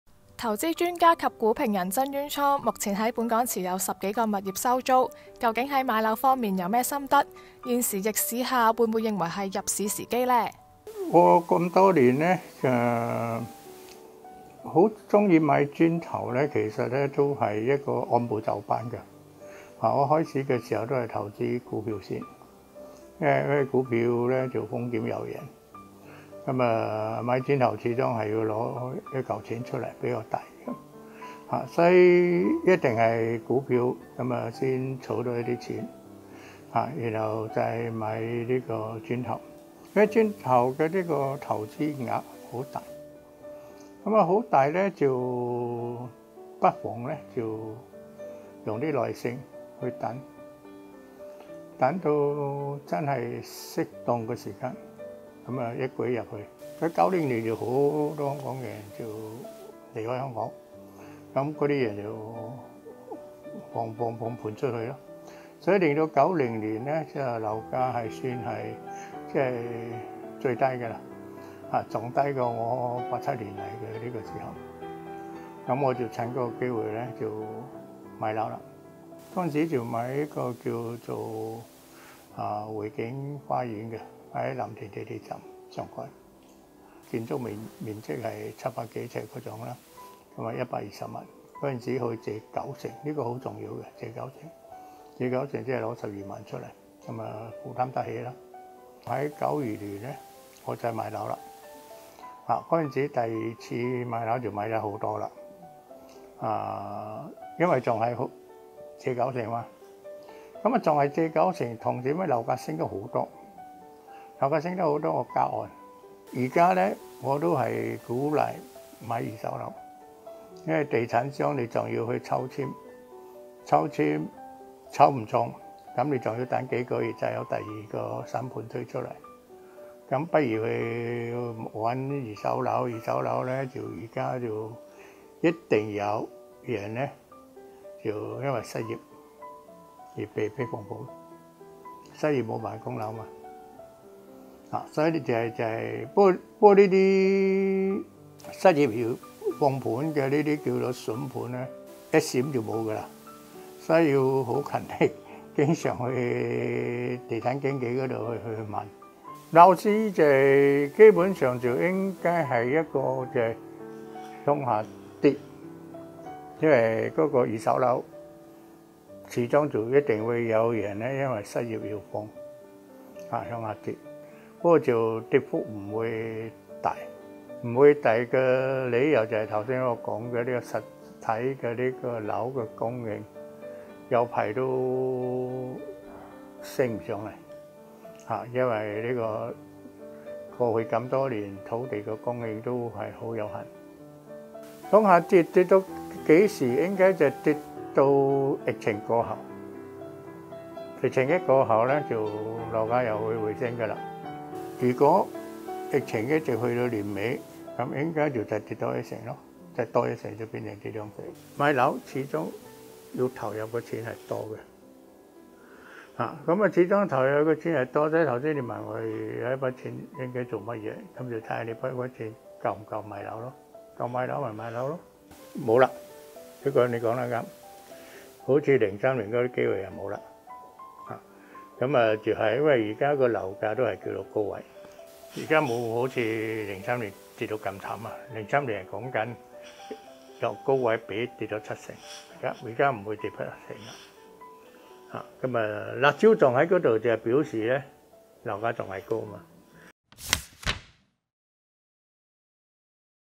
【名人入市】曾淵滄把握90年移民潮 12萬首期滙景3房上車
投資專家曾淵滄接受本站專訪時表示，若疫情持續，預計樓價至年底前將跌約1成，但不會重現「沙士」價。。。